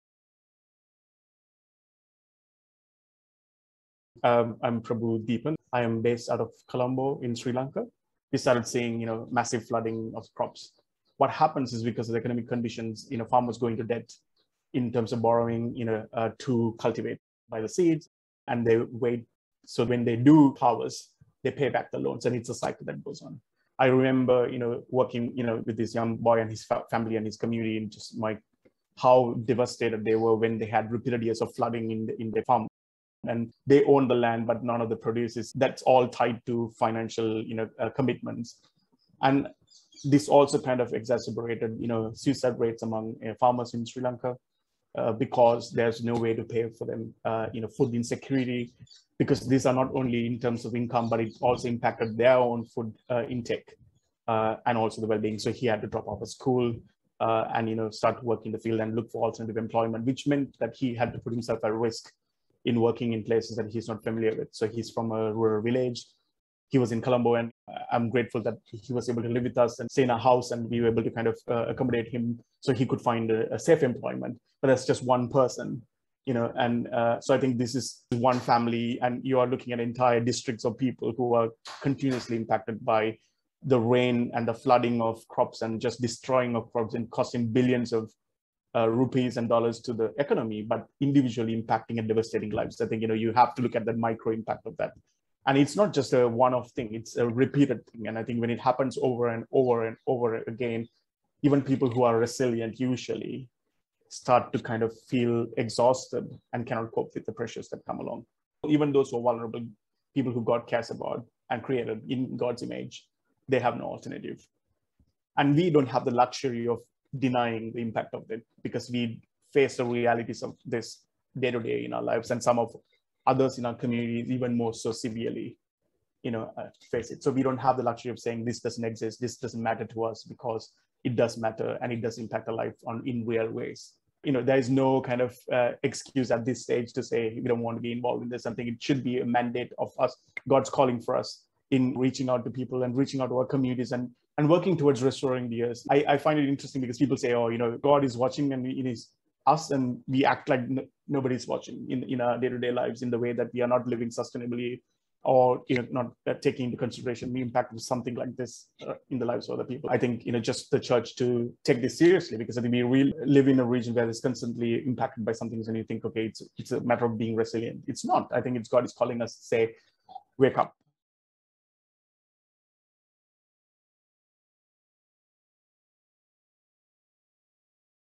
Frontlines of Climate Change: Sri Lanka
In Sri Lanka, the climate crisis is an undeniable reality: after years of destructive flooding, many people have become trapped in debt. Prabu Deepan highlights that climate change is not a one-off event but an ongoing crisis that is making life even harder for local communities.

You can get involved with prayer and action on the climate crisis with Tearfund's Reboot campaign